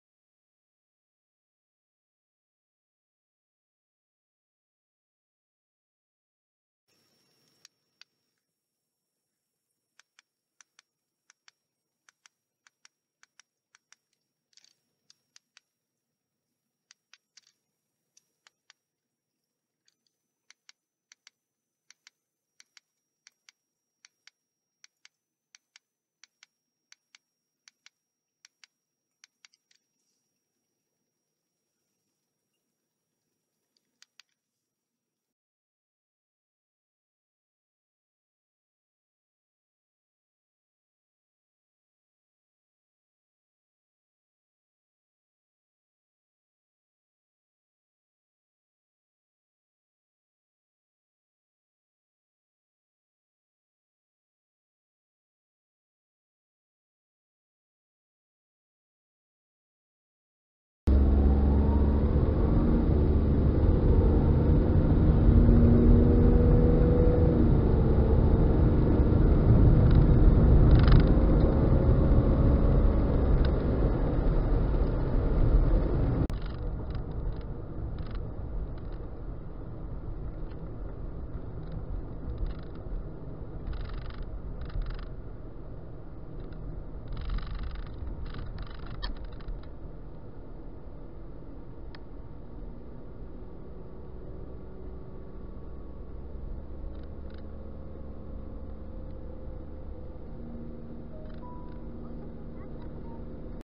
激安 !? 2500円の ドライブレコーダー 検証してみた。 TKYH2 ？ 2.5インチ 300万画素 らしい。
説明
2500円のドライブレコーダーを検証します。
赤外線ライト6個
2.5インチモニター
8倍デジタルズーム
撮影サイズ　1280*960 1280*720 720*480 640*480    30FPS
写真サイズ　640*480～12M

バッテリー 600mAh
シガーソケットで充電（差しっぱなしにしてます）

記録メモリ　SDカード（32Gまでとのこと）microでも対応してます

出力形式　AVI

初ドライブレコーダーなんで
これが良いのかどうかわからないのですが。